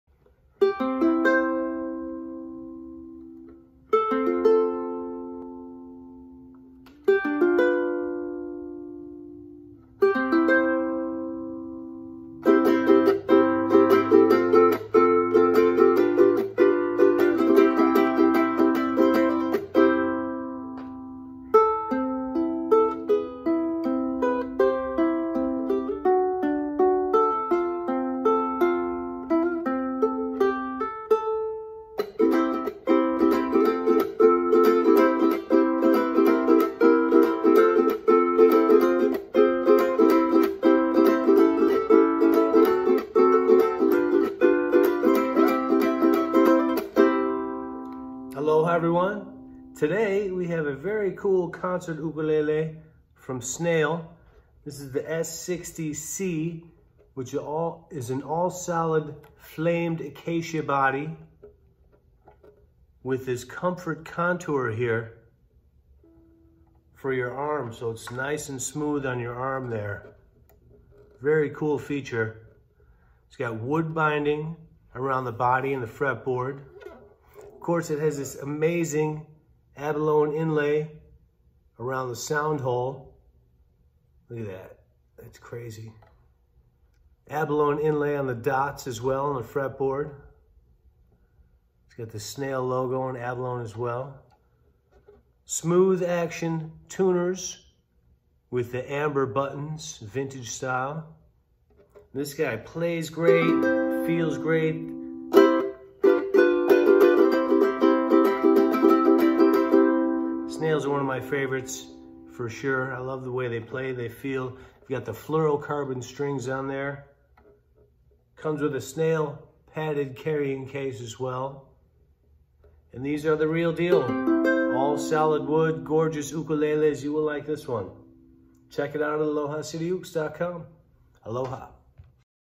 Snail S-60C Concert Ukulele Demo/Review at Aloha City Ukes
Snail S60C Concert Ukulele